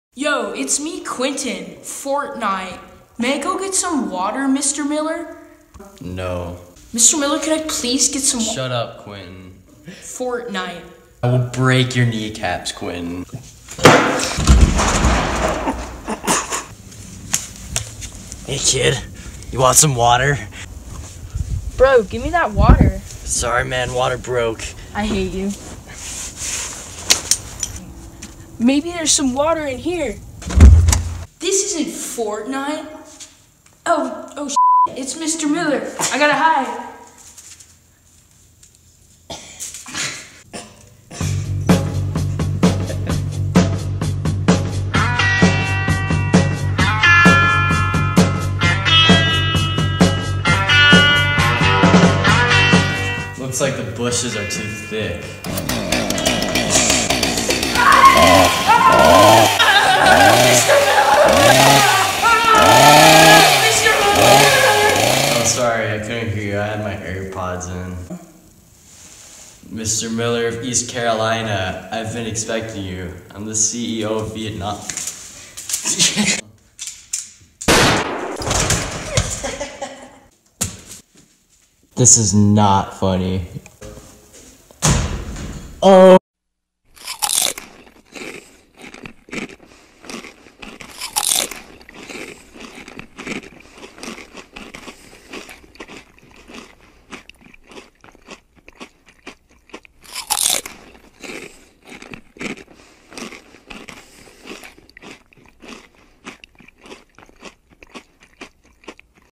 Quinten's Water Quest in Vietnam
When Mr. Miller doesn't let Quinten get water, he becomes arid and goes to vietnam.

This video is not for kids because it involves violence, death, 13+ content, parody, war, guns, and inappropriate language.